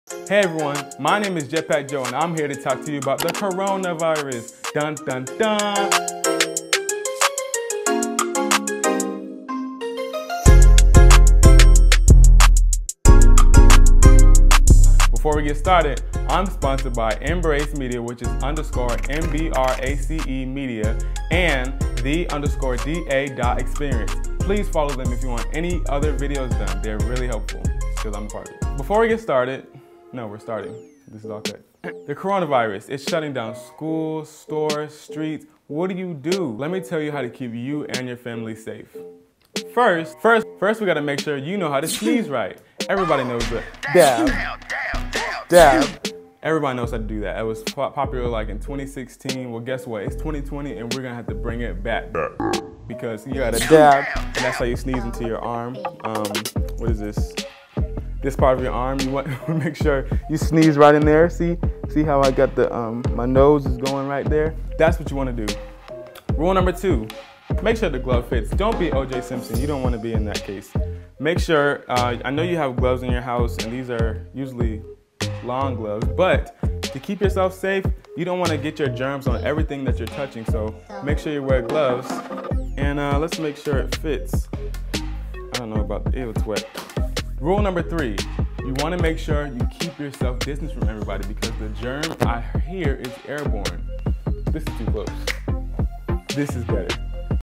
How to survive the Coronavirus and flattenthecurve
We helped @jetpackjoe to put 3 tips together to survive the Covid19 aka the Coronavirus. As CNN is consistently giving us updates we are just as concerened about our health and safety which are of utmost importance right now and we wanted to put something together to encourage people to practice social distancing. Remember this is just a chapter of our lives not the whole story. Be encouraged things will get better. Please share this with your community.